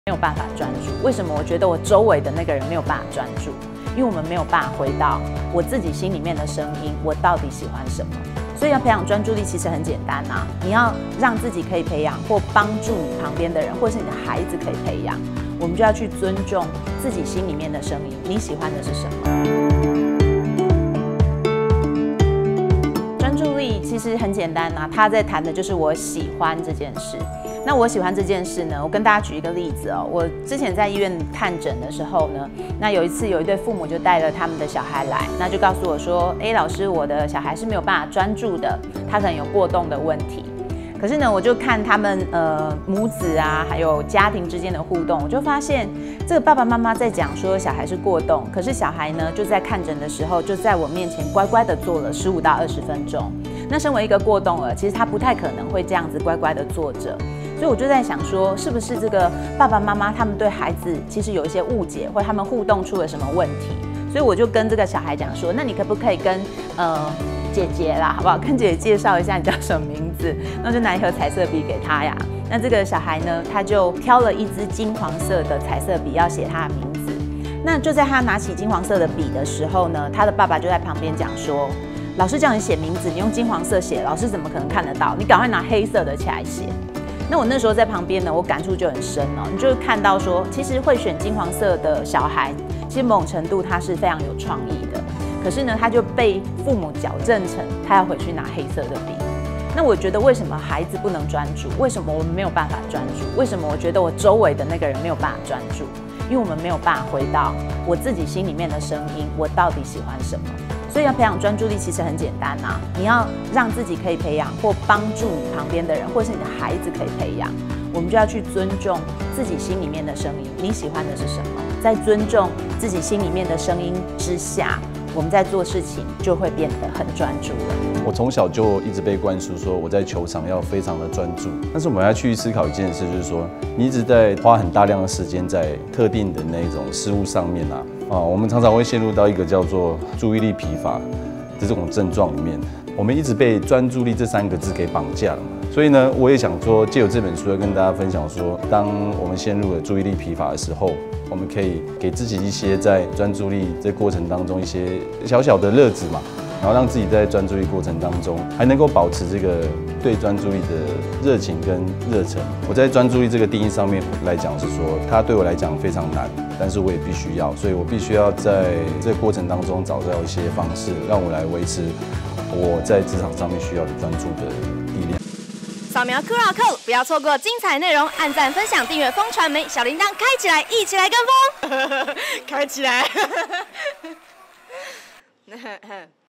想找回專注力應該先這樣做，高效心智鍛鍊顛覆你的想像
研究指出，專注力是一種強大的心智力量，能大幅提高個人的成就與成功機會。其中「自我覺察」正是形塑專注力的重要元素，除了學習適應外在環境，更重要的是由內而外的了解自己。

心理學家告訴我們，想找回專注力首先應該傾聽自己內心的聲音，找到熱愛的事物與目標，更能有效提升專注力，進一步獲得滿足與成就感。心智鍛鍊對人的影響超乎想像，一起來聽聽他們的觀點吧！